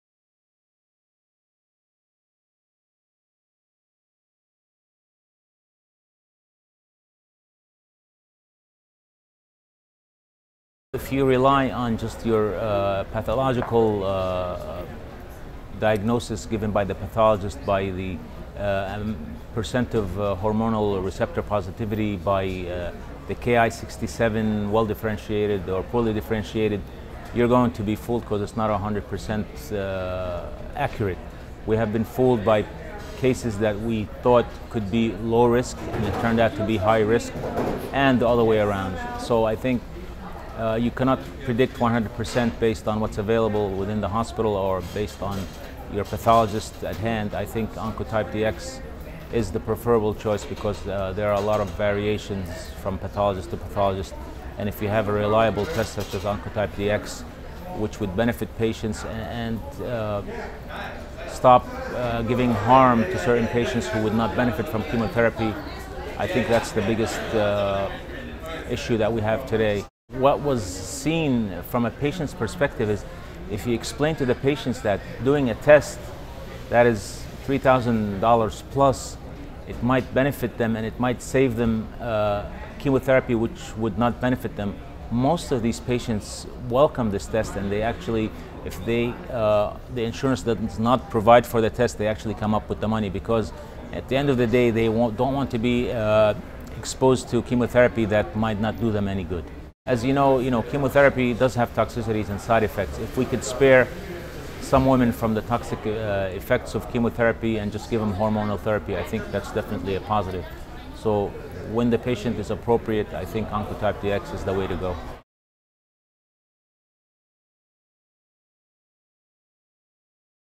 Refining the therapeutic approach for breast cancer through genomic profiling
At the 2015 American Society of Clinical Oncology (ASCO) Annual Meeting, Mohammed Jaloudi, MD, of Tawam Hospital, Al Ain, United Arab Emirates, discusses the value of genomic profiling for individualising and improving adjuvant therapy for breast cancer patients. More recently, multigene assays, such as the Oncotype DX® Breast Cancer Assay, are being considered in addition to classic clinicopathological tools to guide treatment decisions for breast cancer patients.

This content is supported by Genomic Health, Inc.